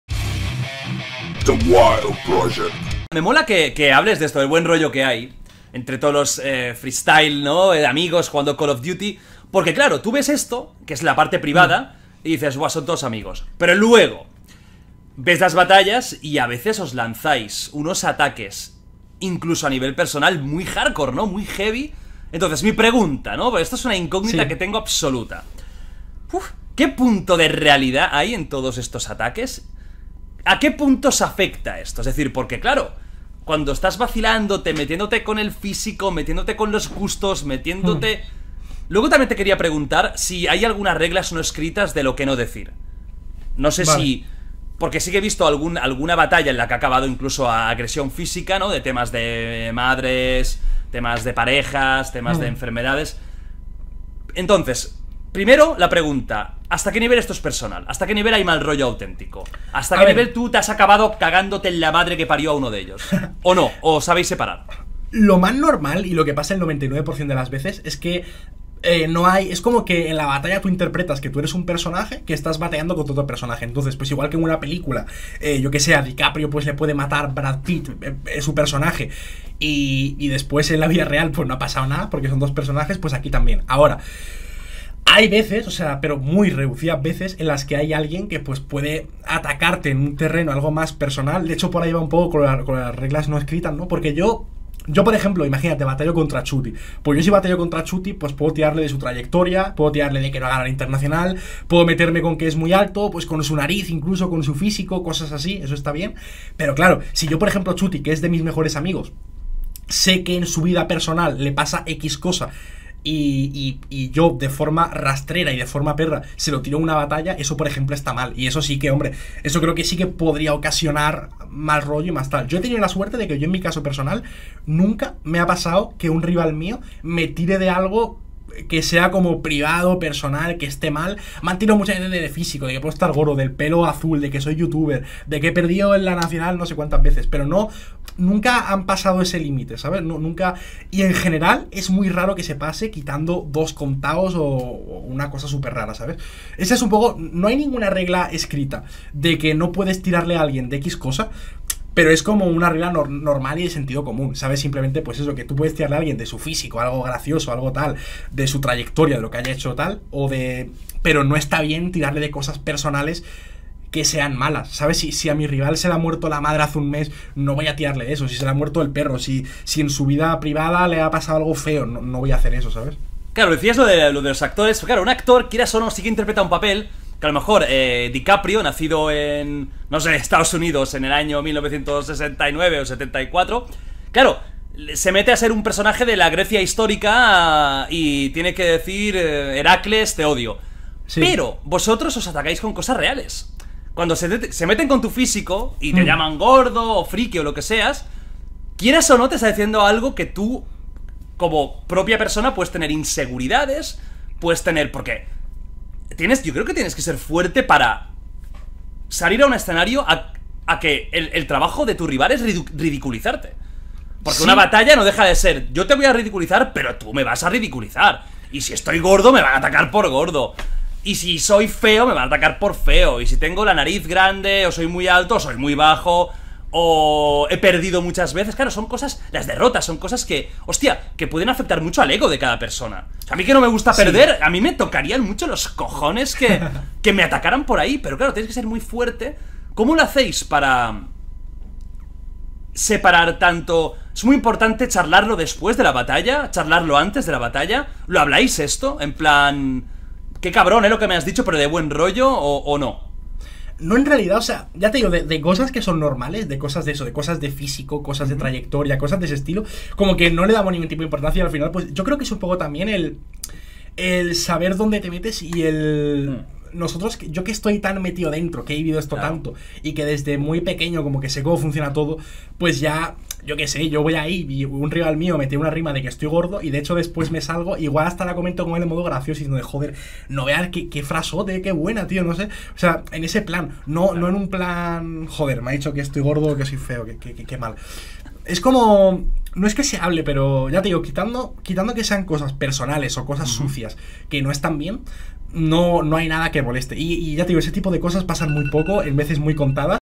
¿Cuánto de verdad hay en los insultos en las Batallas de Gallos? FORCE nos lo cuenta | Jordi Wild
La verdad sobre el tema.